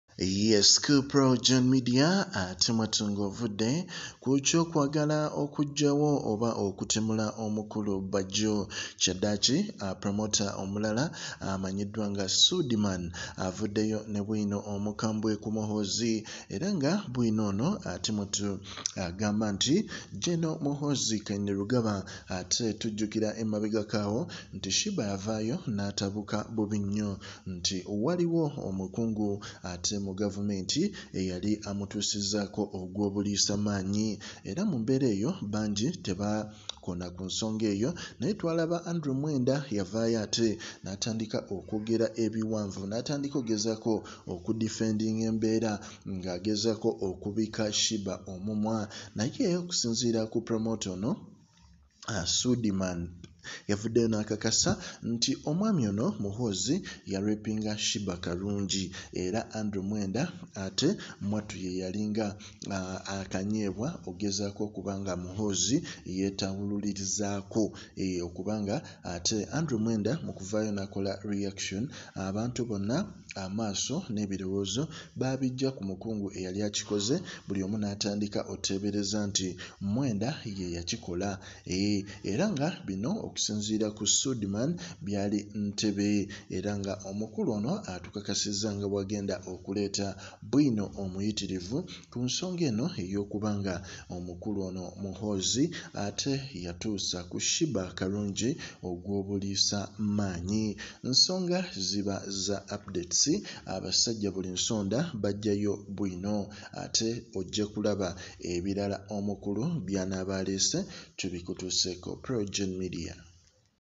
Ebyama Muhoozi okwaata sheebah Bwino Azuuse Aze akwaata abayimbi ngateri amukuba kumukono
Gen Muhoozi Kainerugaba raped Sheebah Karungi, Andrew Mwenda is a escape goat
This took place in Entebbe I have more details coming soon